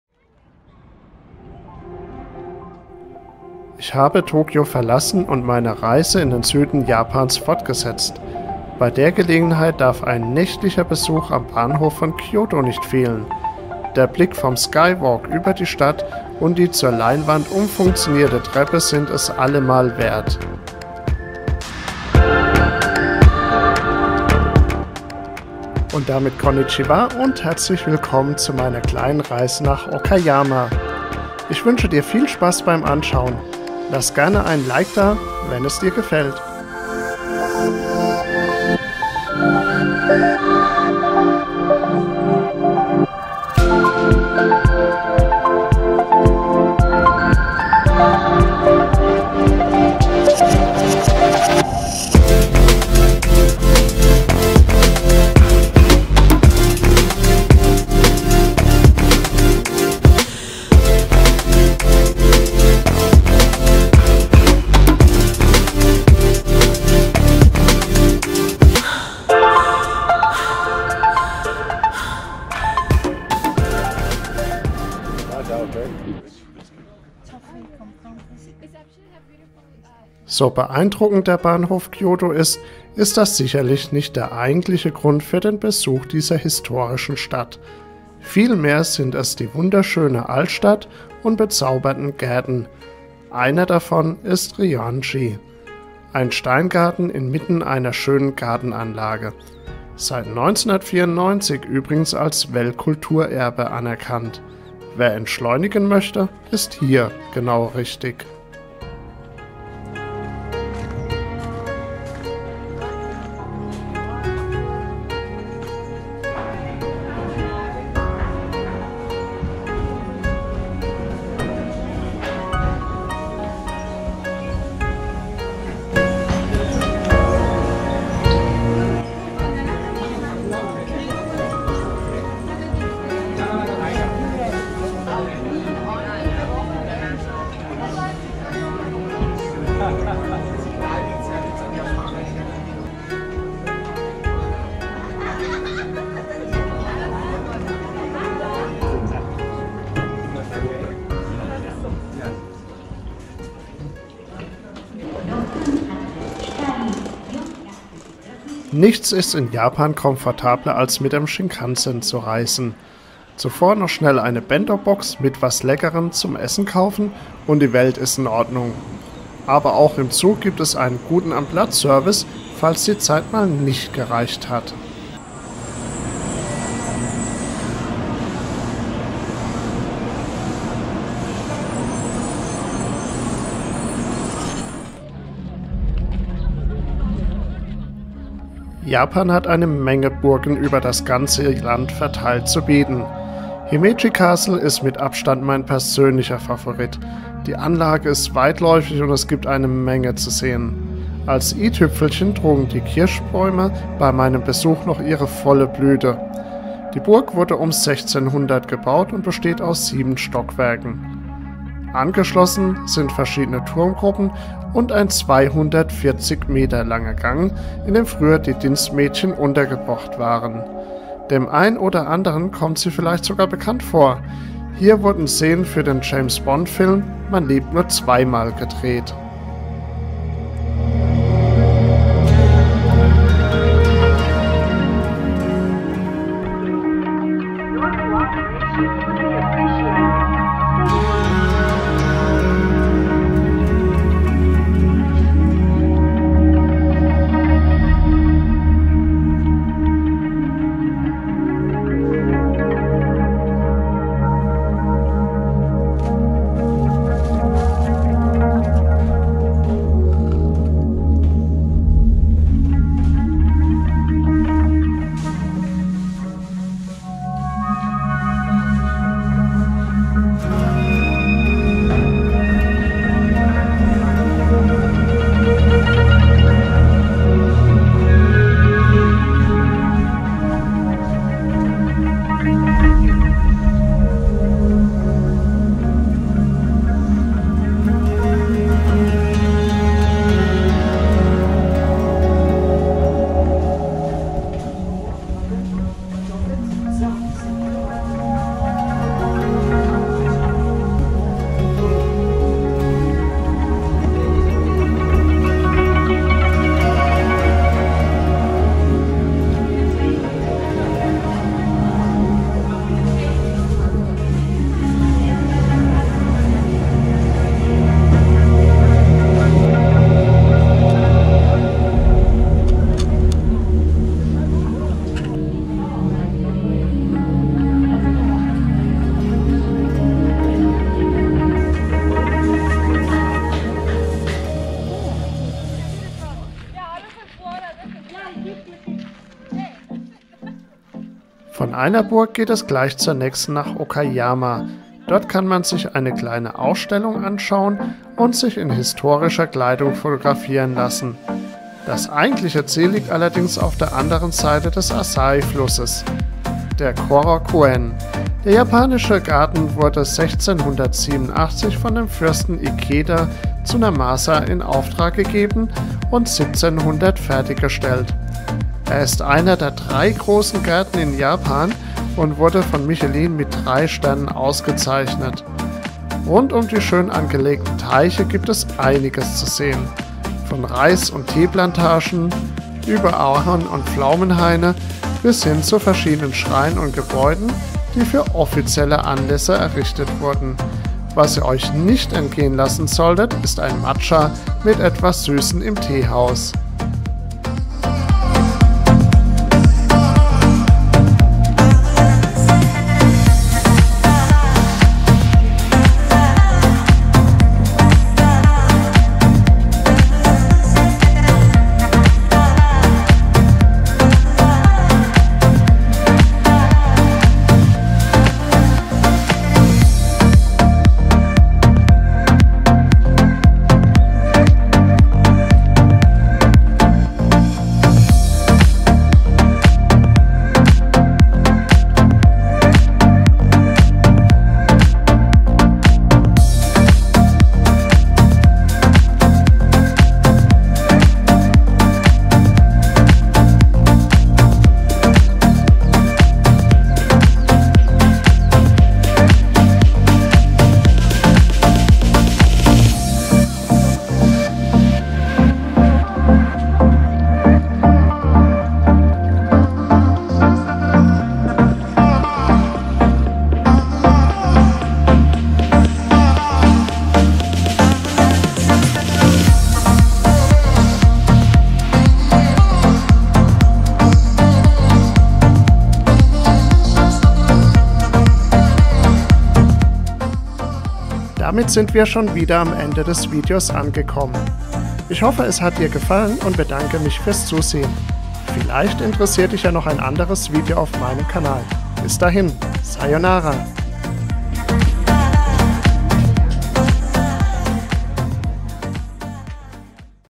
Von Kyoto mit dem Shinkansen nach Okayama und die Frage was James Bond mit Himeji Castle zu tun hat
Hallo zusammen,

in dem kleinen Video nehme ich euch mit in verschiedene Gärten und  Burgen in Japan. Davon gibt es so einige.

Nach einem kurzen Rundgang durch den Bahnhof Kyoto, besuchen wir den Steingarten Ryoanji.

Danach geht es mit dem Schnellzug Shinkansen weiter in Richtung Okayama. Dort schauen wir uns den Korakuen, einer der Top 3 Gärten in Japan.

Auf dem Weg dort hin muss unbedingt ein Zwischenhalt im Himeji Castle eingelegt werden. Ein absolutes muss. Die Burg und der Garten sind wunderschön. Und ganz nebenbei auch ein Drehort eines James Bond Films. Na, schon eine Idee? Im Film erfährst Du die Antwort.

Viel Spaß beim Anschauen. Über einen Like, Feedback und Fragen freue mich :-)